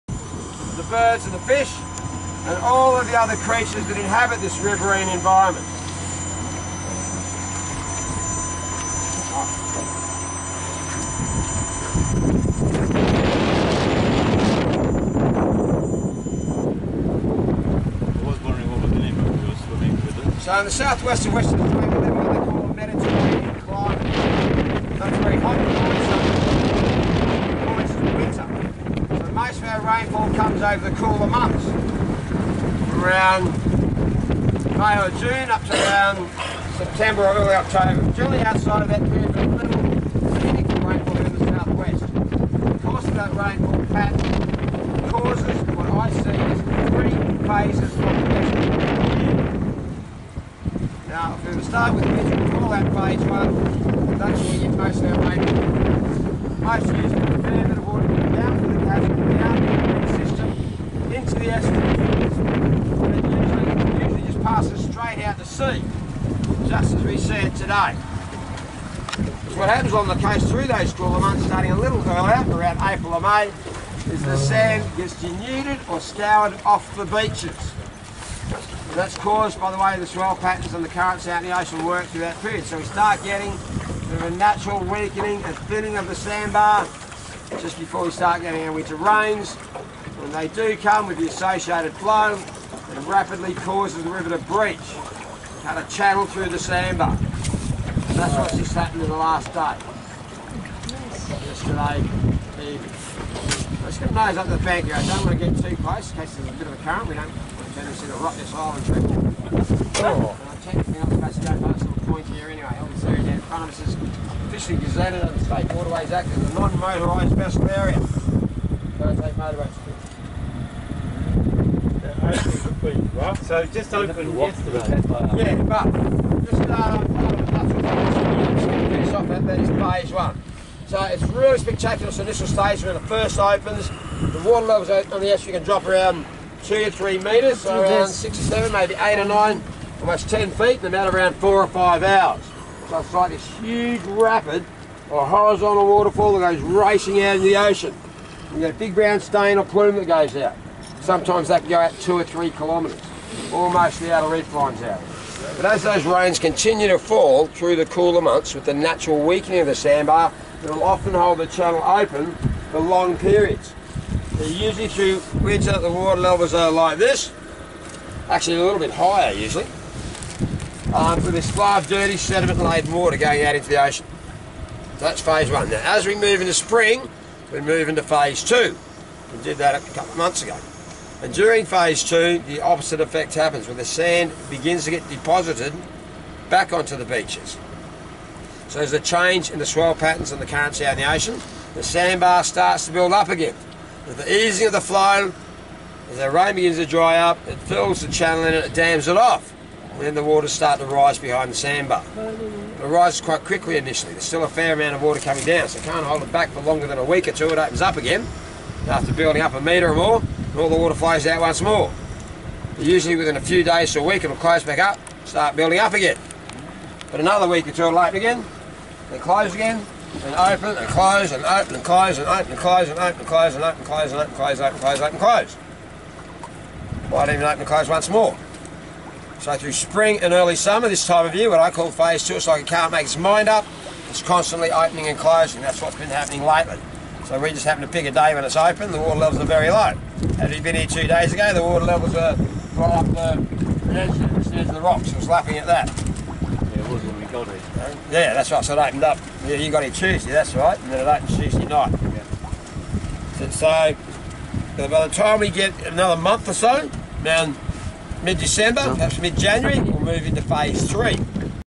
River Cruise at Moore River, The Lorikeet Centre Recovery Camp
Vlog done from a boat, tour guide talking in the background. Explaining the beauty around this place.